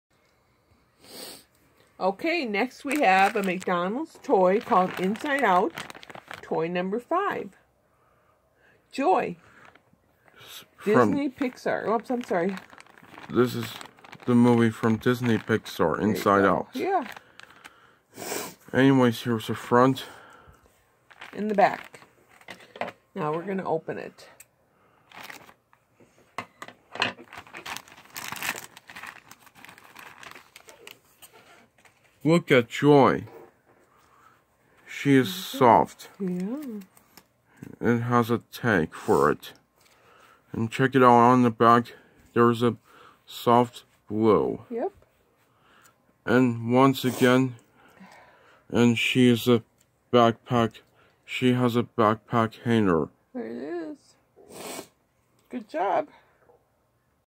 Disney Pixar's Inside Out McDonald's Toy #5 Joy Unboxing
Here is an unboxing of Disney Pixar's Inside Out McDonald's Toy #5 Joy.
Original Video Record Date: March 3, 2021